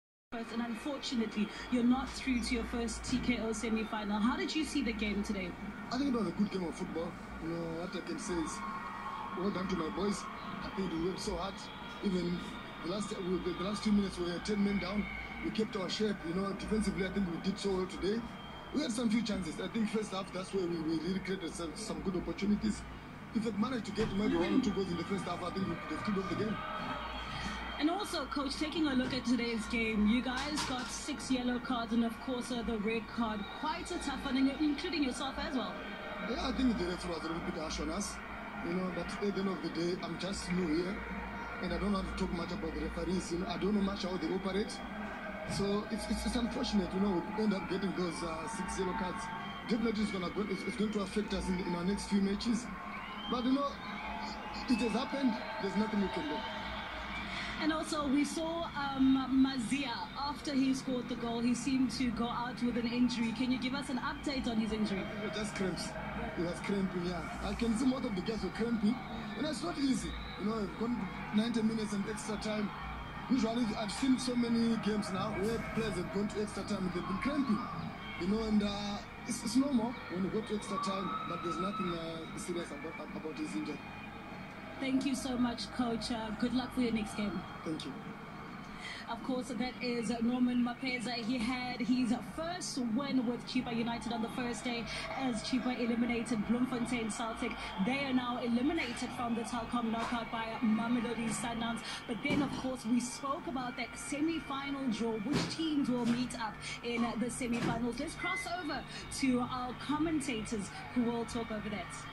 TKO2019 Chippa United 2 (3) - 2 (4) Mamelodi Sundowns (Coach Norman Mapeza interview)
SS4 interview with Coach Mapeza